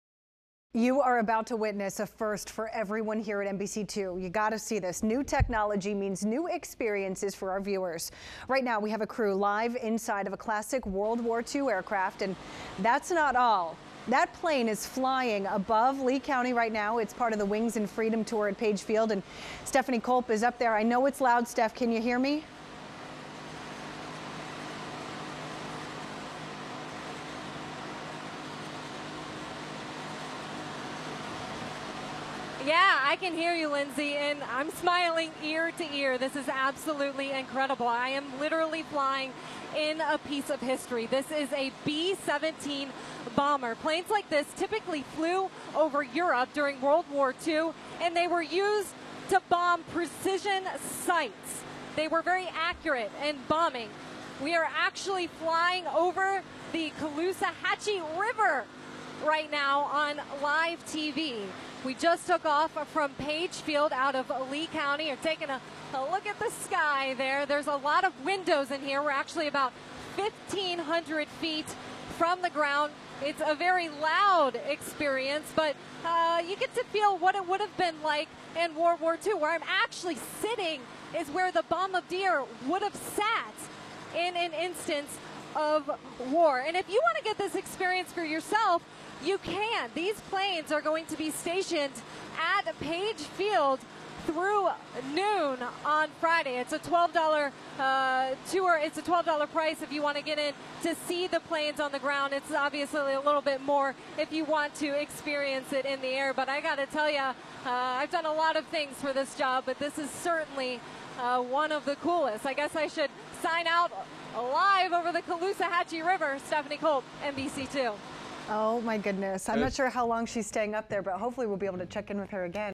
Live Shot from a B-17 using Cellular-Bonding Technology
This is a live shot for our 4pm newscast that we did using cellular-bonding technology.  The latency (delay) was a bit longer than normal due to the fact we were 1,500 feet in the air flying over Lee County, Florida.